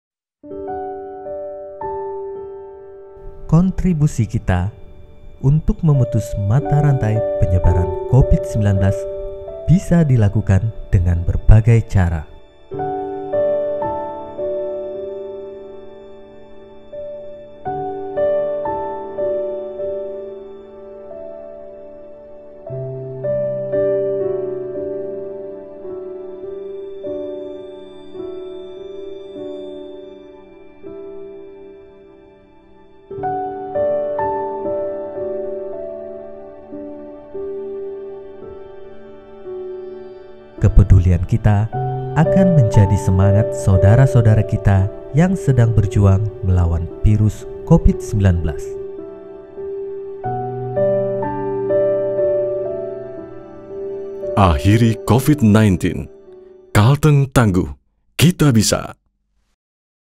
Peduli Terhadap Pasien Isoman
Sepasang suami istri memasak makanan untuk diberikan kepada tetangganya yang menjalani isolasi mandiri

Kontribusi kita untuk memutus mata rantai penyebaran covid-19 bisa dilakukan dengan berbagai cara. Kepedulian kita akan menjadi semangat saudara-saudara kita yang sedang berjuang melawan virus covid-19.